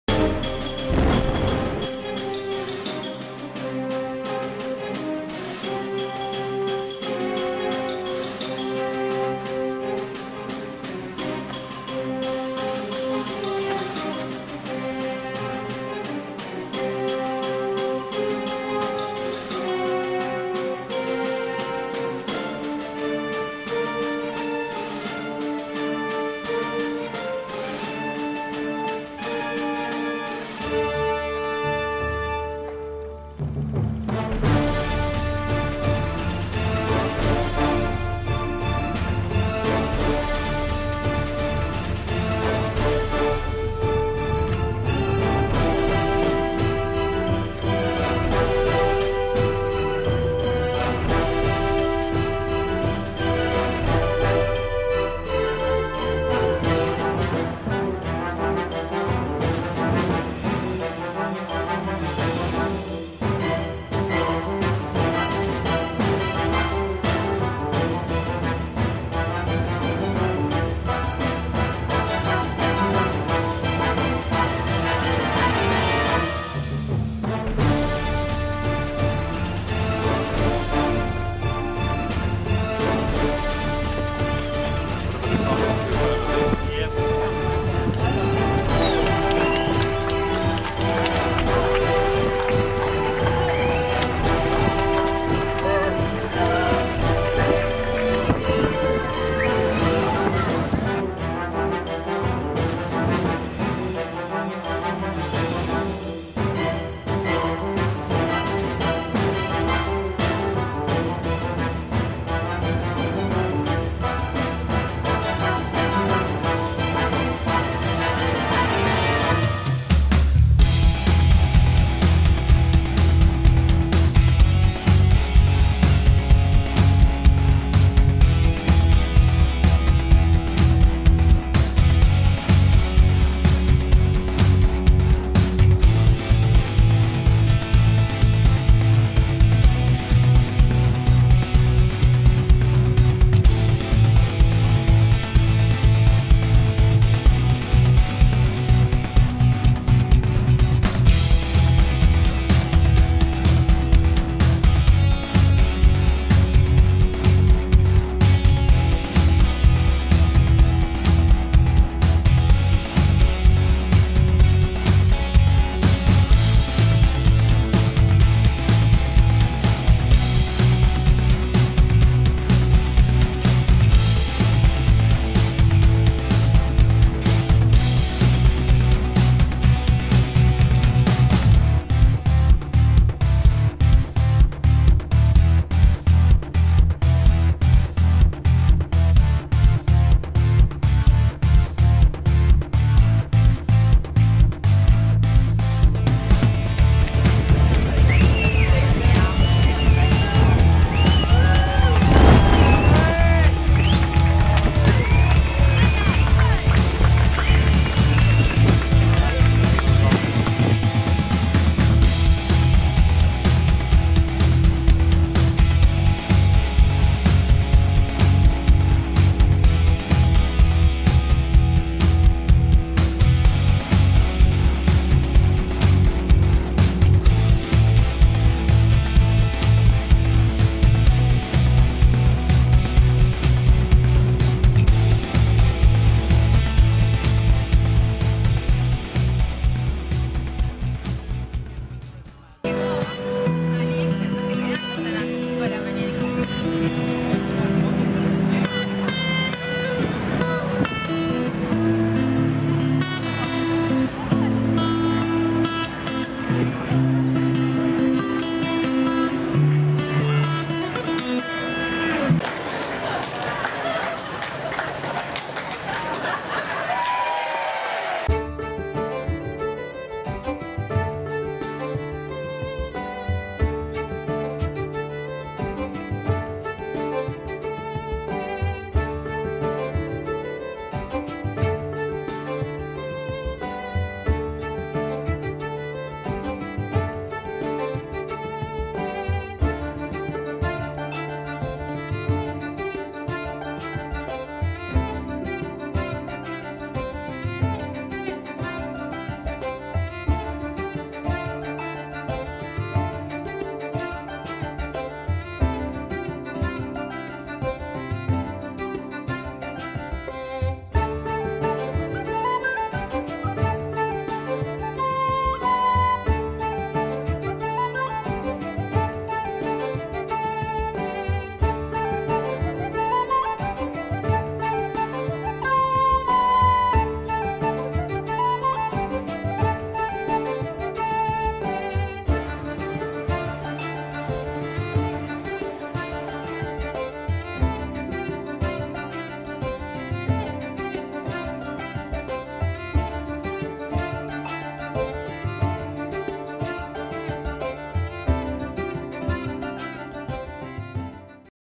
Volvo
Volvo Race 17 March 2012 on St Patricks Day in Auckland. The Viaduct Basin was humming after the race. Filmed with Canon 7D